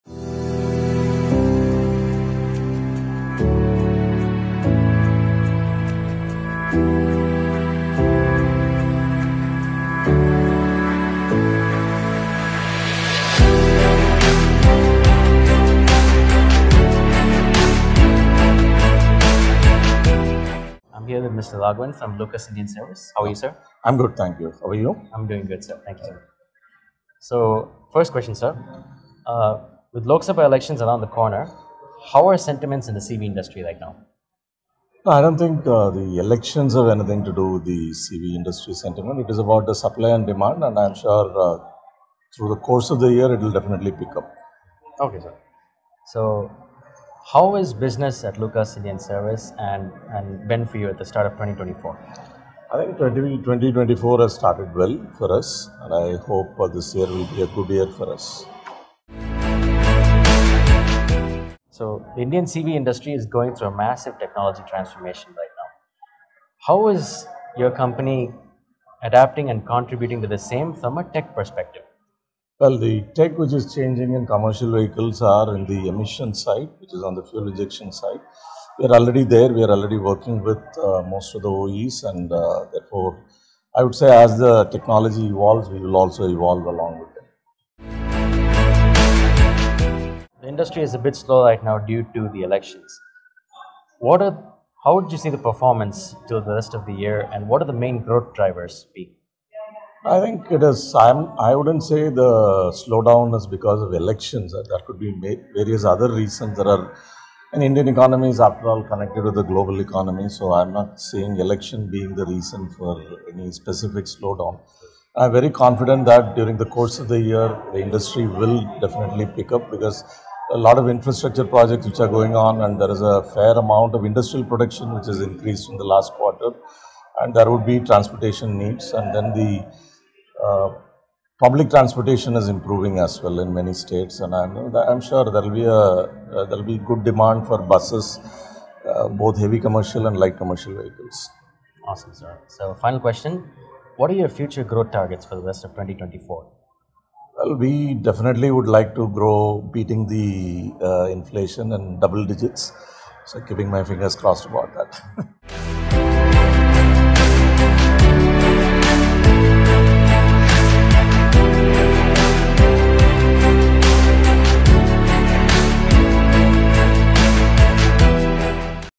CII Commercial Vehicle Summit | Exclusive With Raghavan N, President, Lucas Indian Service Limited
An Exclusive Interview with Mr. Raghavan N, President, Lucas Indian Service Limited at the CII - Commercial Vehicle Summit - 2nd Edition with the theme of “Viksit Bharat powered by CVs.

The Commercial Vehicle Summit brings you closer to the transport solutions of the Future, New customers, Market and Regulatory Requirements which challenges the operators as well as developers permanently. The summit brings together top-class experts from the automotive & automotive supply industries (OEMs, Suppliers & Technology providers), Government agencies and Research Institutions to discuss about the best practices and new approaches.